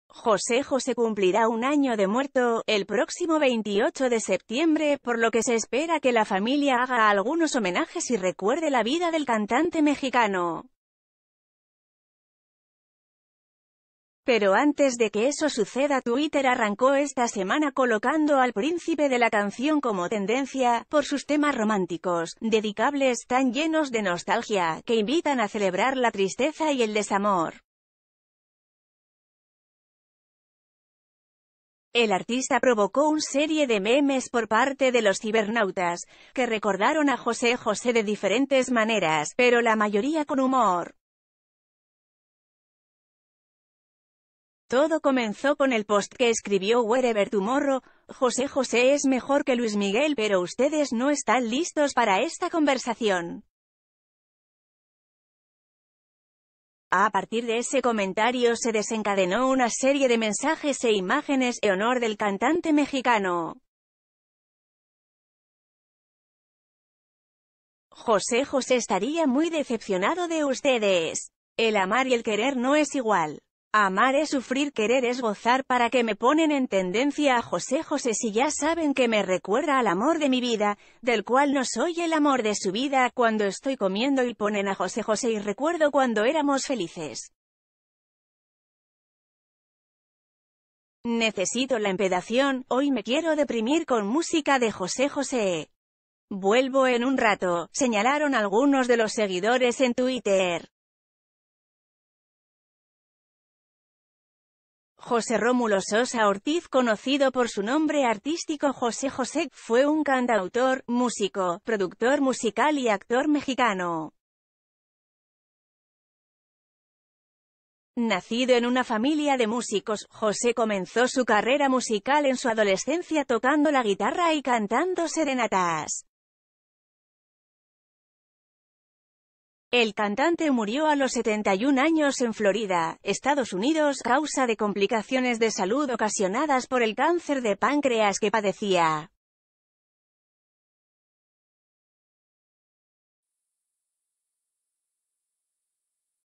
José José trae una trágica pérdida para todos, el mensaje revela
El fallecido cantante se vuelve tedencia y los usuarios lo recuerdan con algunos mensajes y memes.